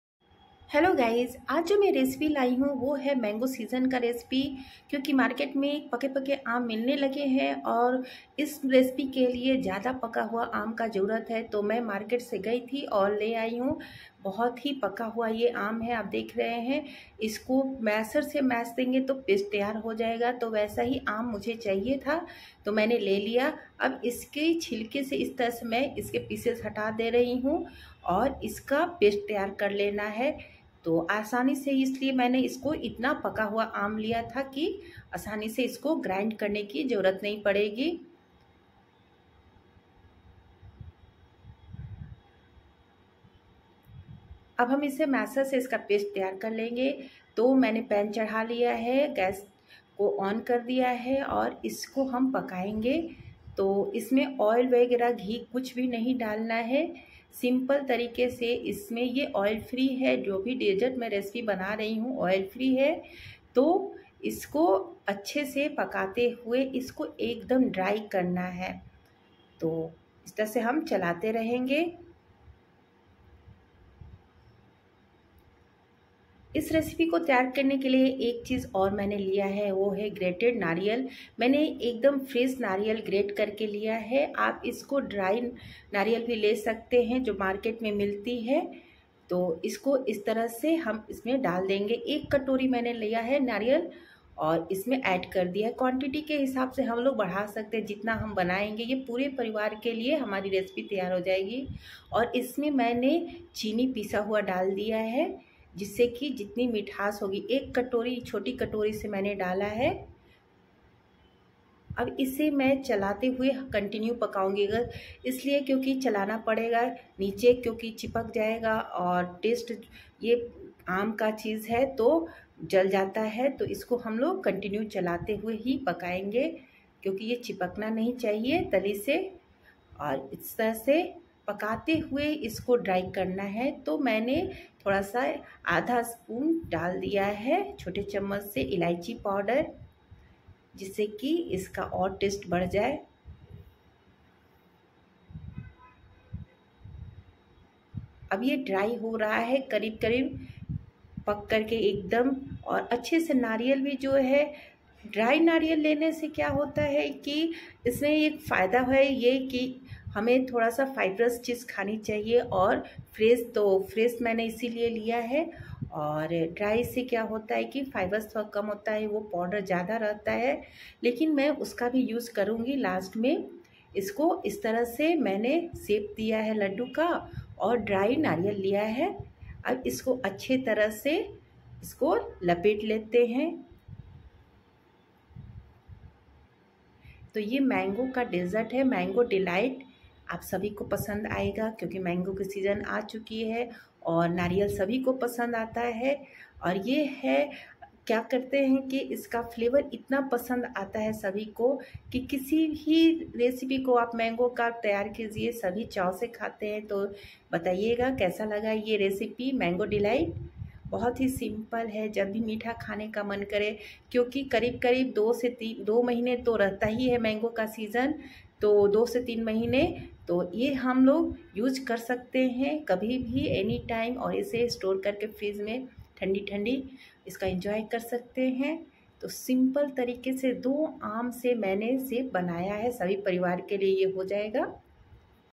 दो पकाआम से पूरे परिवार के लिए रेसिपी एक बार बनाये पूरे महीने खाये | mangorecipe | aamrecipe | ladoo
00:00 Intro to Mango Delight Recipe 🥭
00:11 Selecting the Perfect Mangoes 🥭
01:02 Oil-Free Cooking Method 🍳
01:35 Adding Fresh Coconut for Flavor 🥥
02:19 Enhancing Taste with Cardamom 🌿
03:06 Shaping the Mango Dessert 🍬
03:49 Enjoying the Seasonal Treat 🎉